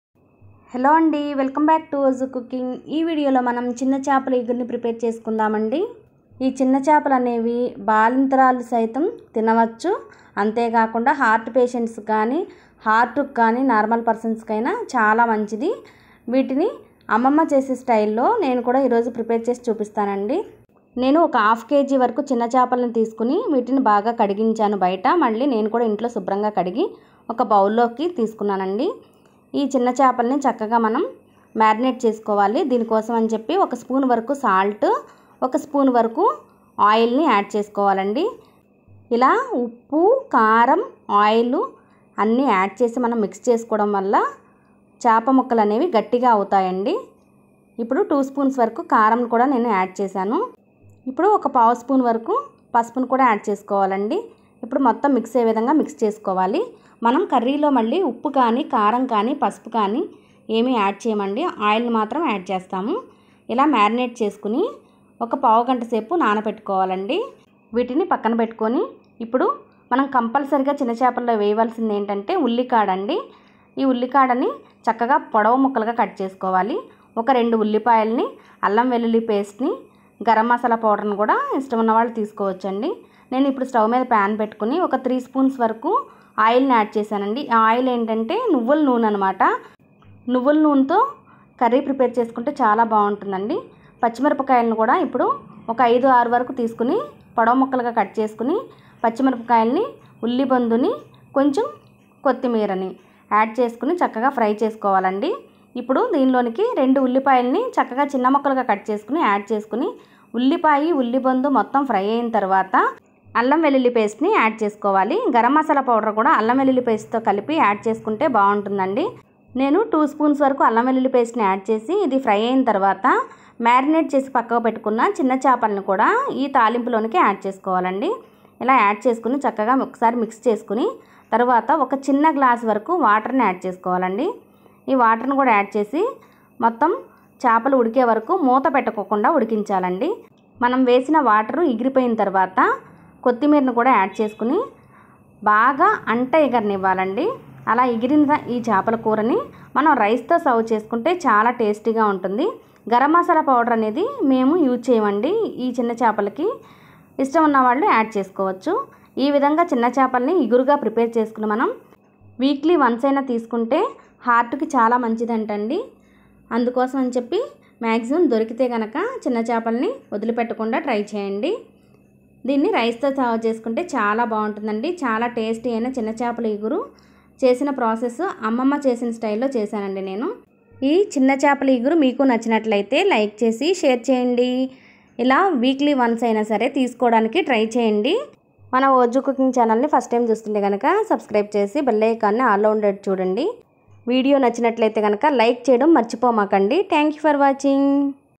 నువ్వుల నూనెతో చిన్న చాపల ఇగురు అమ్మమ్మ స్టైల్ లో చేస్తే చాలా బాగుంటుంది బాలింతల సైతం తినవచ్చు||fish
నువ్వుల నూనెతో చిన్న చాపల ఇగురు అమ్మమ్మ స్టైల్ లో చేస్తే చాలా బాగుంటుంది బాలింతల సైతం తినవచ్చు||little fish

ఈ వీడియో మీకు నచ్చినట్లైతే లైక్ 👍👍👍👍👍👉👉👉 చేసి షేర్ చేయండి